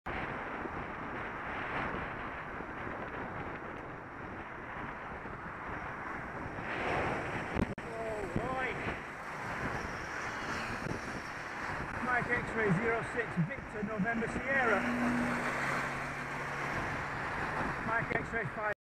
MX06VNS & MX58DYH Come on guys; @FirstManchester drivers are better than this!
Location: Broad Street, Salford (Westbound)
Date & time: 4.45pm on 30th July 2014